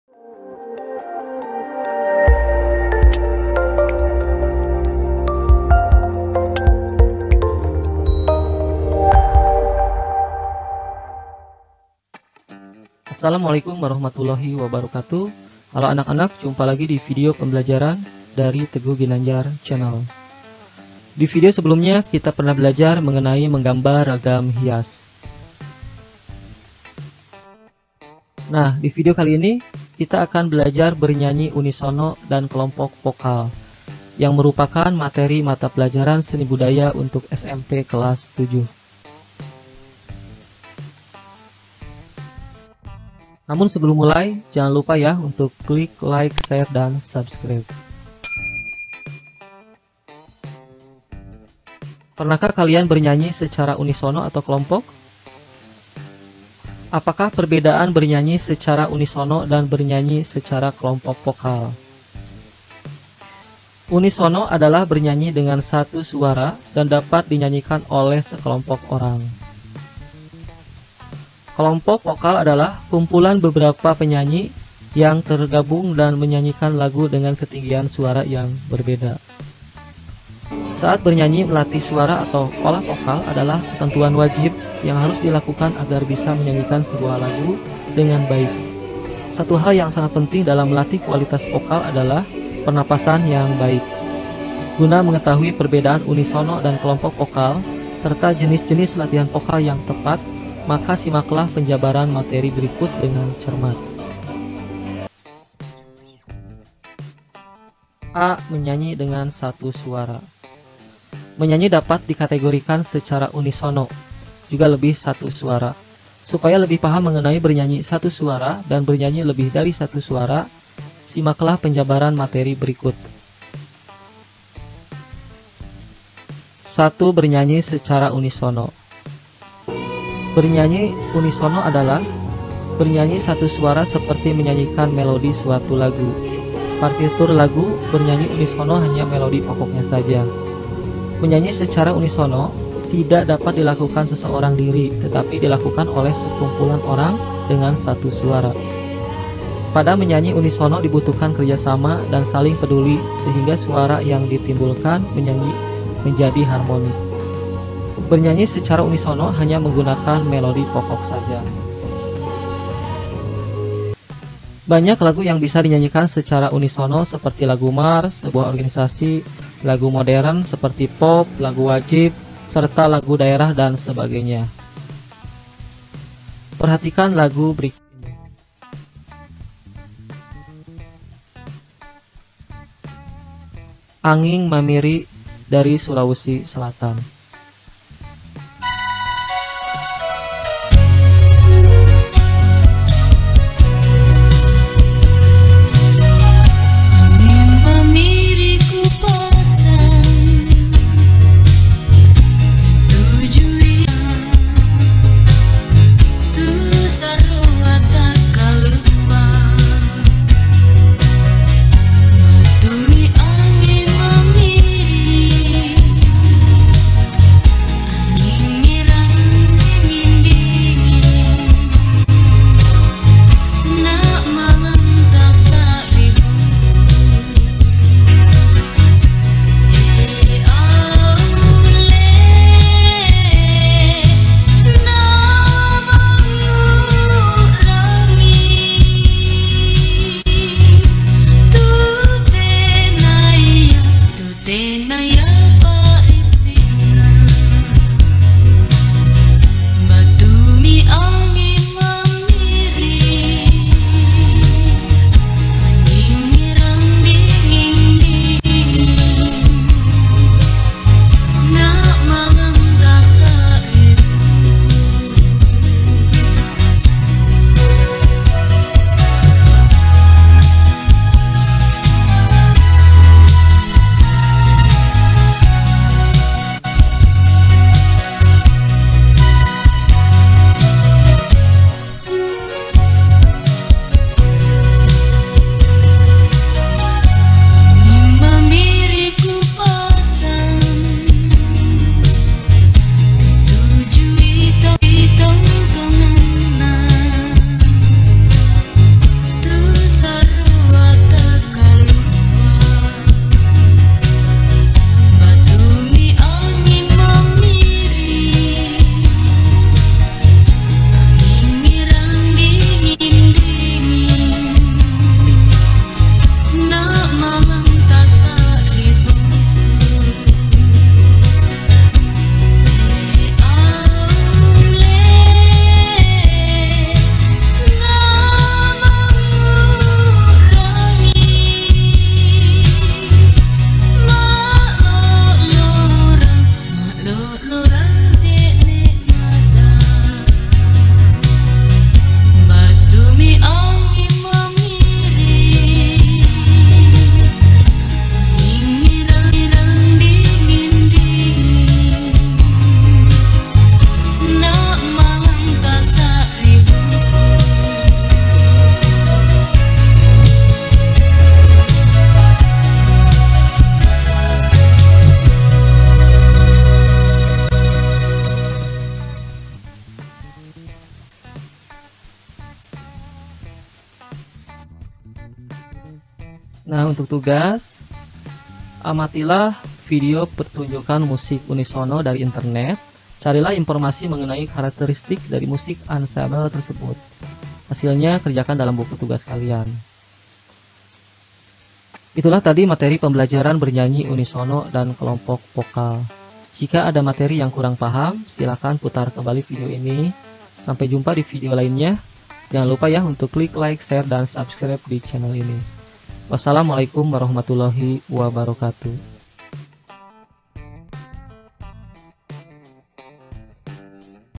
Materi Pembelajaran Bernyanyi Unisono dan Kelompok vokal, Seni Budaya Kelas 7.
Materi Pembelajaran SMP Seni Budaya Kelas 7, Bernyanyi Unisono dan Kelompok vokal.

Nah pada materi ini kita sudah masuk pada bab 2 yaitu materinya membahas tentang Bernyanyi Unisono dan Kelompok Vokal.

Tujuan Pembelajaran
Setelah memperlajari materi tersebut siswa diharapkan dapat.
1. Memahami konsep bernyanyi
2. Memahami dasar bernyanyi dengan dua suara atau kelompok.
Silahkan kalian tonton sampai habis

Background Musik
Monks